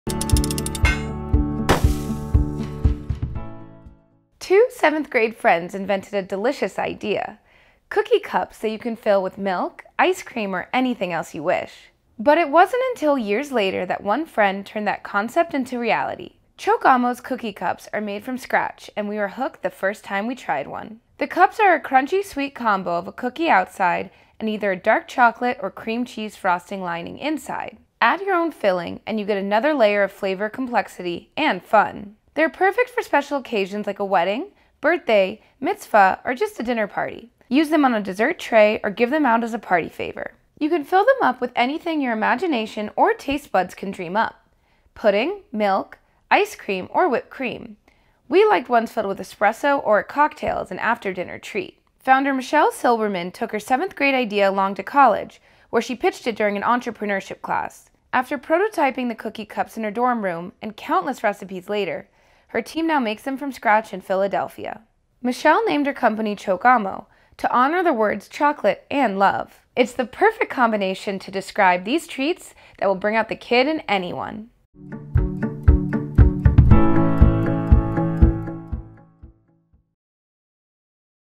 Serve dessert . . . in dessert.
Back in the 7th grade, ChocAmo Founder Michelle Silberman and her best friend had the idea to combine milk and cookies in one treat—cookie cups. Flash forward to today, and Michelle’s chocolate chip childhood dream is a tasty reality thanks to her college entrepreneurship course.

ChocAmo’s cookie cups are made from scratch in Philadelphia. The cups are tasty as-is, and come in flavors like Chocolate Chipster, a marble cookie with sea salt and a layer of dark chocolate, and Glitz and Glam, a sugar cookie with Madagascar vanilla cream cheese frosting rimmed with silver sprinkles. (Is your mouth watering yet?)

Fill a cup up to take it to the next level of deliciousness—think fillings like ice cream, pudding, espresso, or even a cocktail shot.

Cookie cups serve as an extra special dessert for special birthdays and parties, or are an easy way to make a custom treat anytime you feel like one.